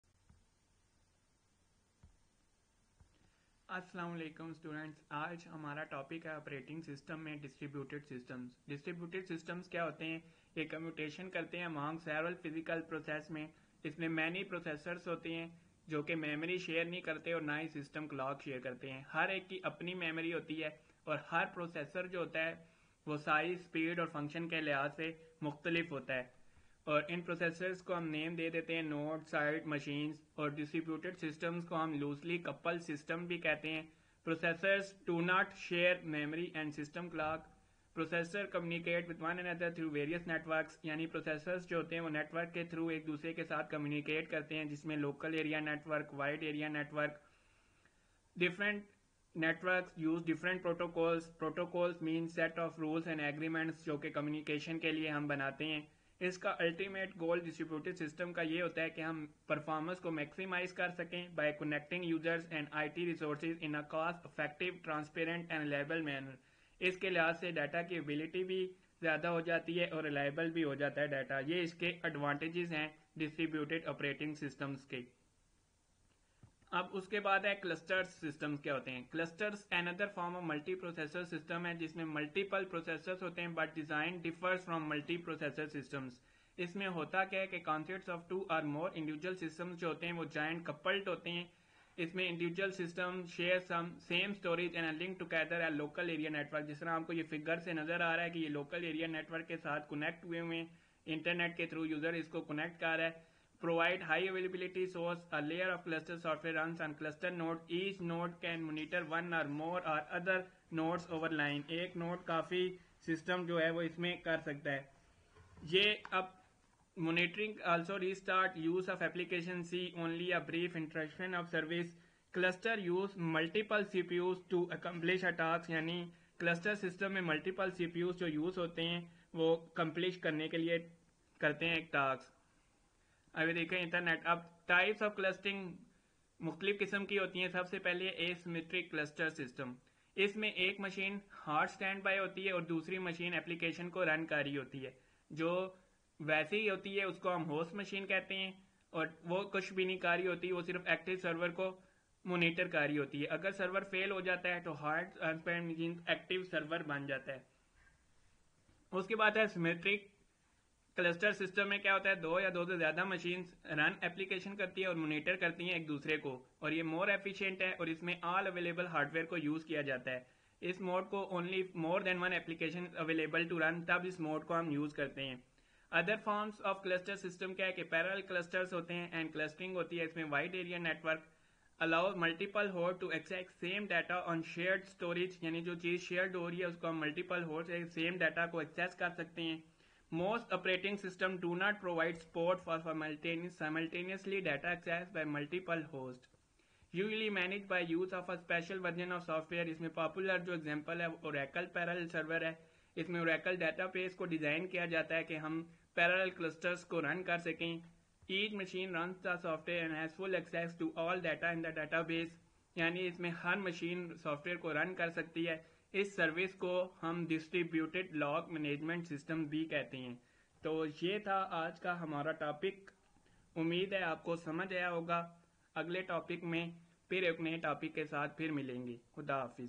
ADP-2 || Operating System || Distributed System || Clustered System || Chapter 1 || Lecture 4
A system that distributes computation among several physical processors
It consists of many processors that do not share a memory or a system clock
Instead, each processor has its own local memory
Processors may vary in size, speed, and function
These processors are given different names such as nodes, sites, machines, and  hosts, etc.
Types of Clustered Systems
Asymmetric Clustered System
One machine is in hot-standby mode, while other machine is running application
Hot-standby host machine → does nothing, only monitors active server
If server fails, hot-standby machine becomes active server
Symmetric Clustered System
Two or more hosts (machines) run applications and monitor each other
More efficient → it uses all available hardware
This mode is used only if more than one application is available to run
Other Forms of Clustered Systems
Parallel clusters and clustering over a wide-area network (WAN)
Parallel cluster → allows multiple hosts to access same data on shared storage
Most OS do not provide support for simultaneous data access by multiple hosts
Usually managed by use of a special version of software
Popular example → Oracle Parallel Server
Special version of Oracle’s database that has been designed to run on parallel clusters
Each machine runs this software and has full access to all data in the database
To provide this shared access to data, clustered system must also provide “access control and locking  service” to avoid conflicting operations
Service is commonly known as Distributed Lock Management (DLM)